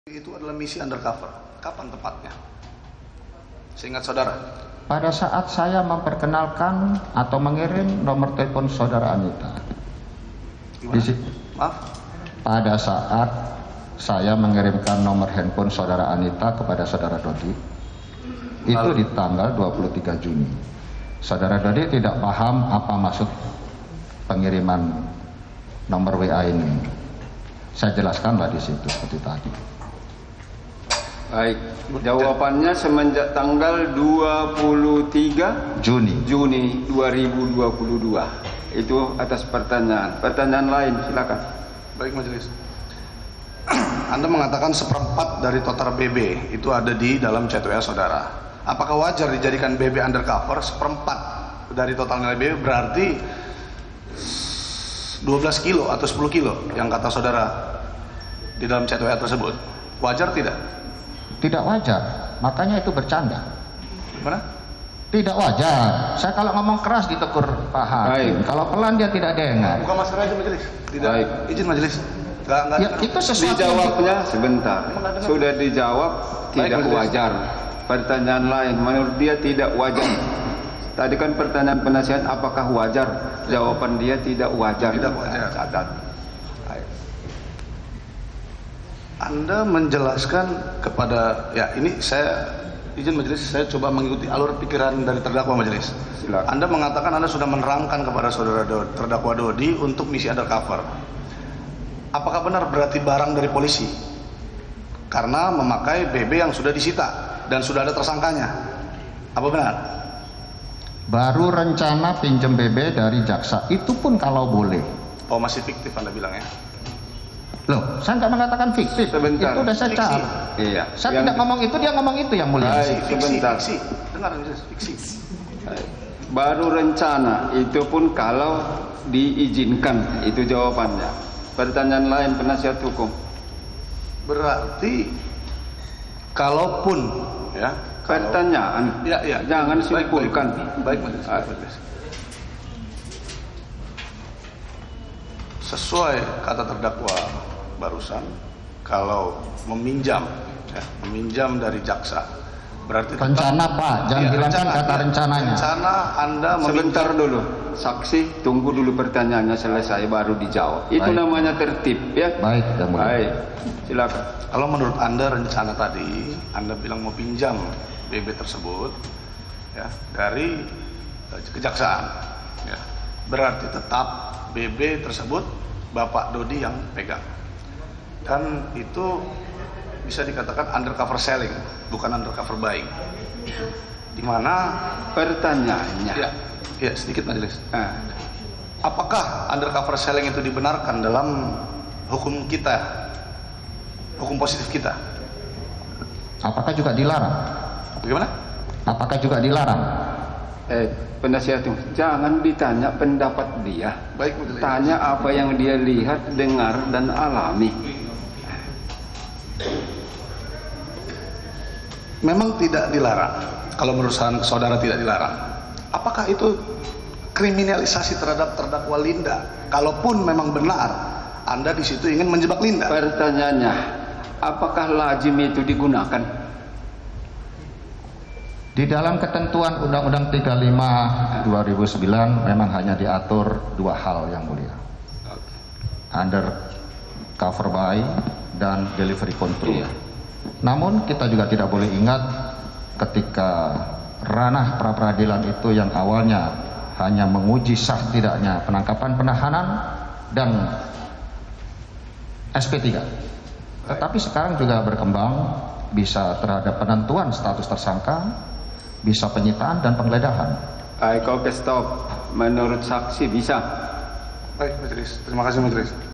BREAKING NEWS - Teddy Emosi saat Menjawab Pertanyaan Kuasa Hukum Dody
MetroTV, Mantan Kapolda Sumatera Barat Irjen Pol Teddy Minahasa menjadi saksi mahkota untuk dua terdakwa peredaran narkoba yaitu AKBP Dody Prawiranegara dan Linda Puji Astuti di Pengadilan Negeri Jakarta Barat.

Dalam kesaksiannya, Teddy berdebat dengan tim kuasa hukum terdakwa Dody saat tim kuasa hukum memberikan pertanyaan.